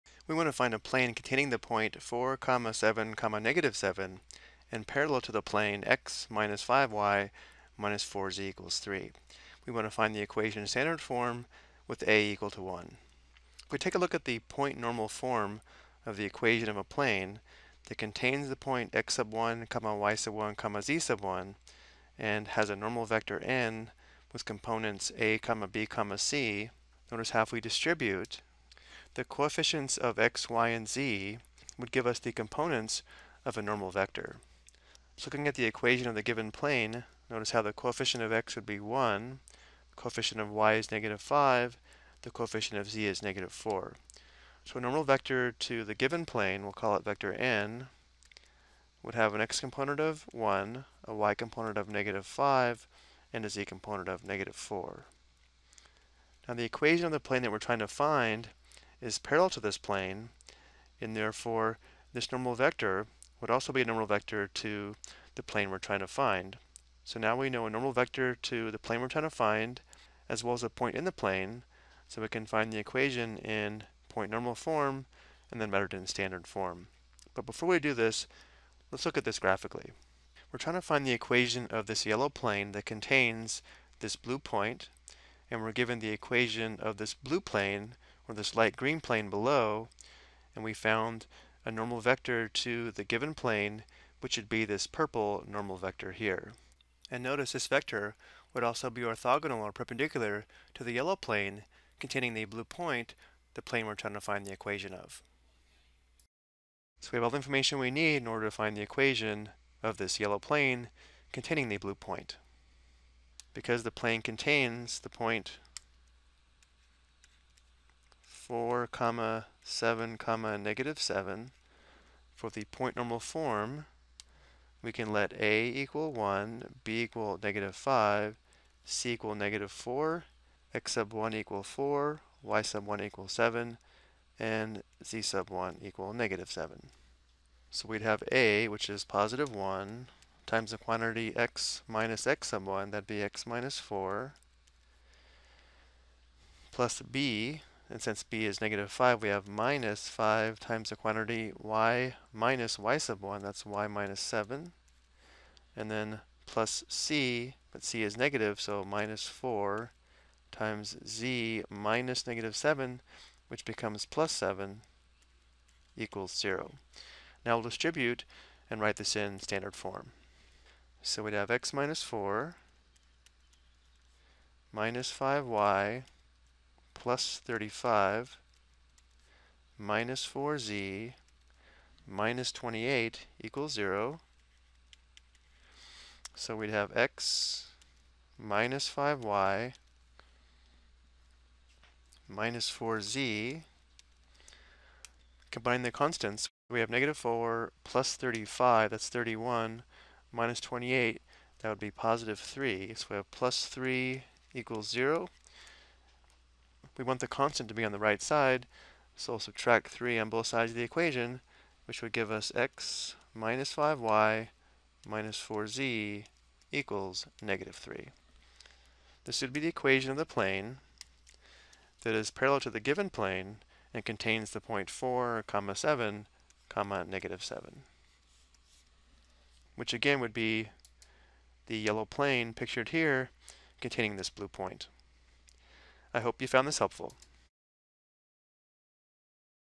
Ex: Find the Equation of a Plane Given a Point in the Plane and a Parallel Plane
This video explains how to find the equation of a plane in standard form given a line the plane contains and the equation of a parallel plane.